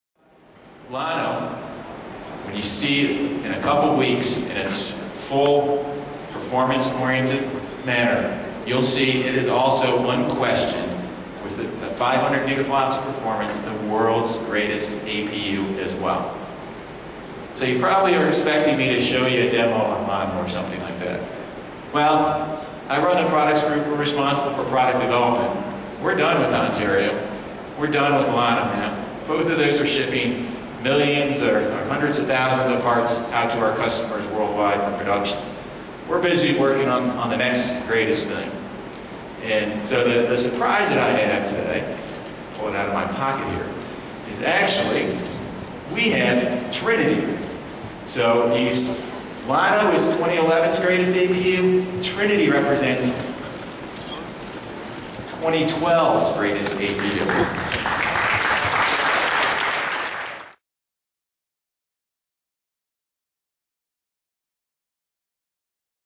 AMD's Next-Gen APU Announced - Trinity
AMD announced the 2012 Fusion based APU roadmap and unveiled Trinity - the next generation performance APU.